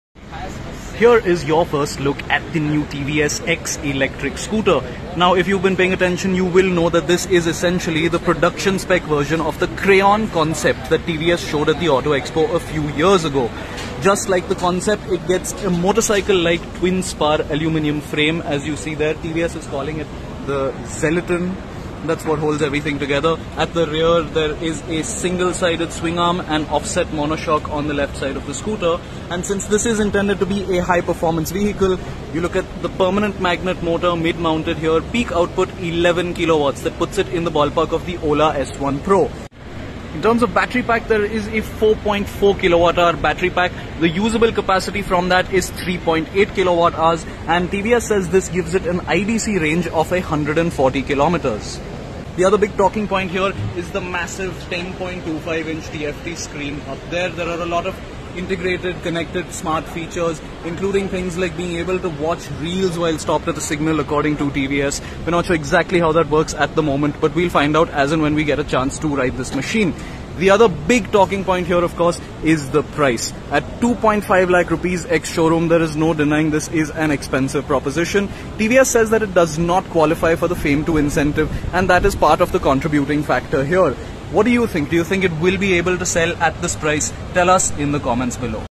TVS X ELECTRIC SCOOTER UNVEIL AT DUBAI BURJ KHALIFA LITTLE WALK AROUND
TVS X Electric Scooter

TVS Motors Company launched its most-awaited electric scooter the TVS X electric Scooter via a Live Event from Dubai today.
This is a Premium Category Electric Scooter That Comes with a Lots of Advanced Features and Modern-day Technology

Here are the All Details About the Latest Launch TVS X Electric Scooter.

Battery-Pack

The TVS X Electric Scooter has a 4.44 kWh dual battery pack which is claimed to be dust and water-resistant.

XLETON platform
The TVS X is built on TVS’s new XLETON platform and gets a maxi-style format. The TVSX is based on a new Xleton frame crafted of aluminum alloy

Charging-time
The TVS X is equipped with fast charging technology and can be charged with either a Smart X Home rapid charger, which takes about 50 minutes to charge from 0 to 50 percent in 50 minutes or using a 950W portable charger that takes 4.5 hours to charge from 0 to 80 percent.

Range and Speed
The TVS X will have a claimed range of 140 kms per single charge. It will also have a top speed of 105 kmph and feature regenerative braking to reduce stress on the battery and extend the driving range.

Digital Features
TVS claims the best aerodynamic efficiency and has ram air cooling technology. TVS claims an industry-first live vehicle location-sharing feature on the TVS X.The TVS X will use an all-new onboard navigation system called Nav Pro. The TVSX features an X-Tilt instrument console that swivels, a segment-first feature.

It will be the first electric scooter in the Indian market to get the largest digital Instrument Cluster at 10.25 inches.

It will come with Three Modes – Xthealth, Xtride, and Xonic. The Xonic is the most powerful ride mode. It gets ABS and cruise control as well.

The TVS X electric scooter comes with a very sporty-looking offset mono suspension at the rear and auto hold function as well.

Other Specifications
0-40 kmph – 2.6sec.

Peak Power- 11kW

105 kmph top speed.

4.44 kWh dual battery pack.

Charging time – 0-50% in under 1hr with fast charging.

Booking and Delivery
Booking of TVS X has been started from tonight and delivery from November onwards. One can book the TVS X at a Booking Amount of Rs. 5000.

Price
The TVS X will be available at a starting price of Rs 2.5 lakh (ex-showroom, Bengaluru).

That price tag makes it the most expensive electric scooter to go on sale in India. As the TVS X costs more than Rs 1.5 lakh, it will not be considered part of the Fame II subsidy. Thus, TVS X does not attract the government’s FAME incentive.

Future Plans
TVS Motors has quickly become one of the most popular electric scooter brands in India with a great product line up. Now, it is planning to launch another new product next month. It is anticipated that it could be the TVS IQube ST, the higher variant of the IQube Series.

It also plans on introducing “a complete portfolio of EVs by the next year and a half.” This shall include multiple products like electric 3W cargo, new variants of their current line ups and much more.